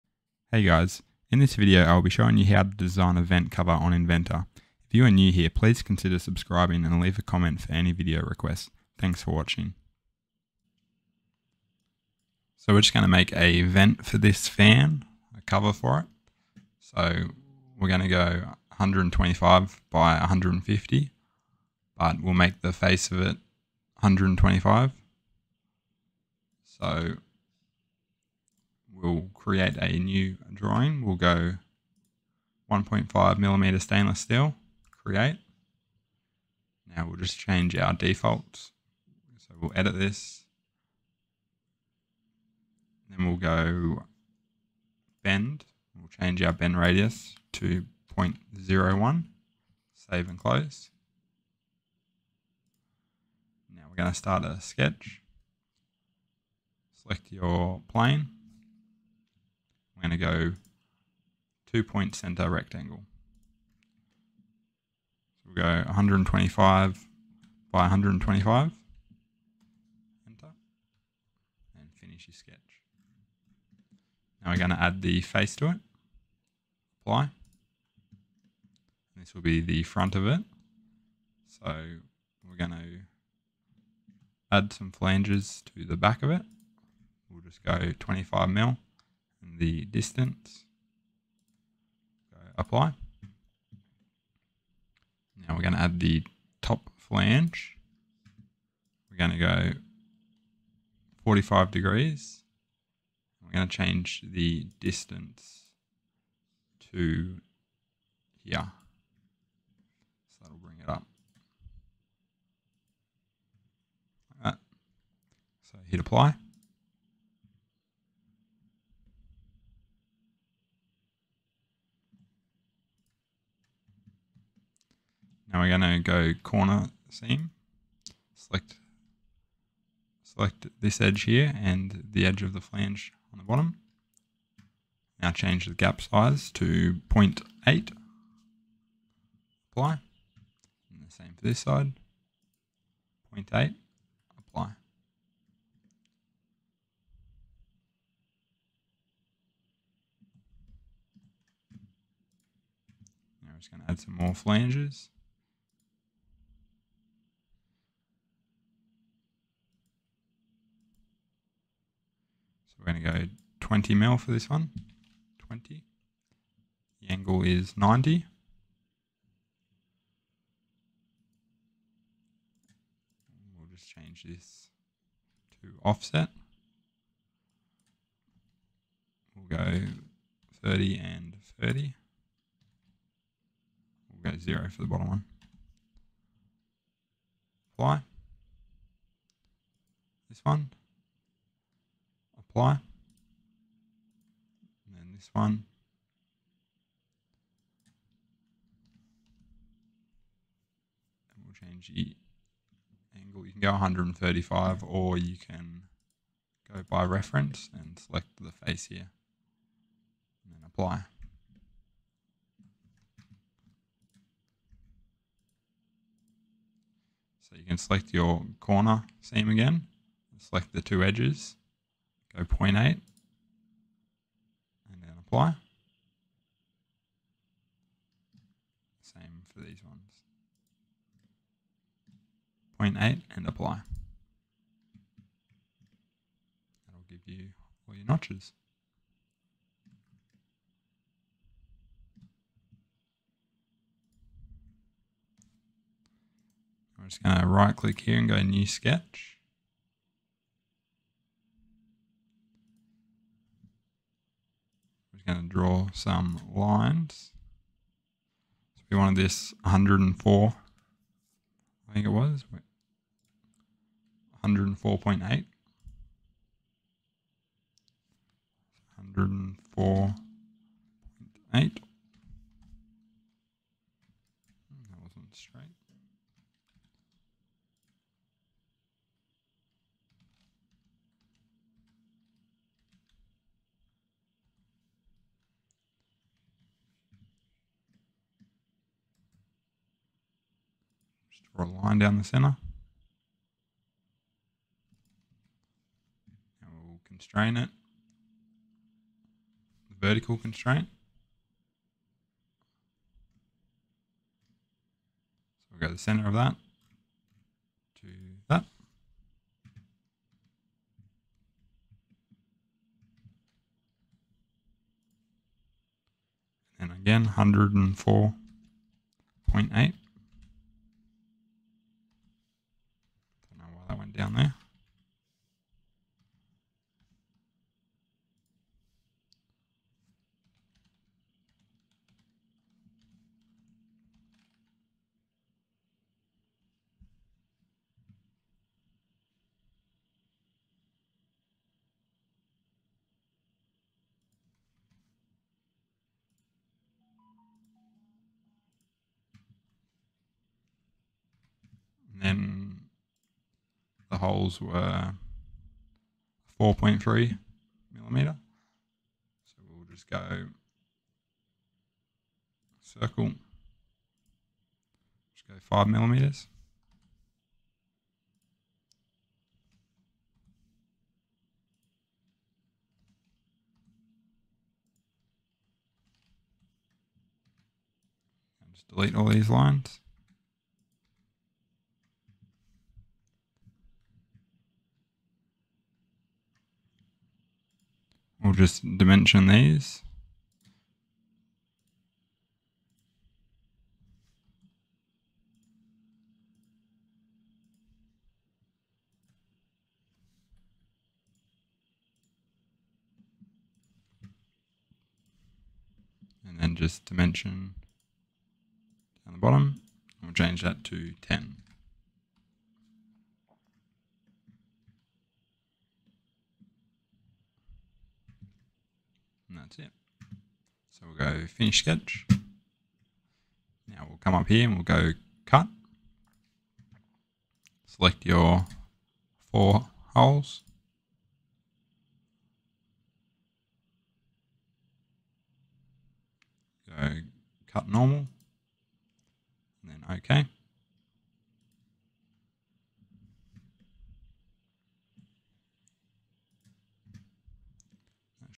Autodesk Inventor 2023 Sheet Metal Tutorial - Waterproof Vent Cover
Hey guys, Another quick step by step beginner guide on how I designed a fan vent cover for a waterproof enclosure

If you have any video requests or ideas please leave them in the comments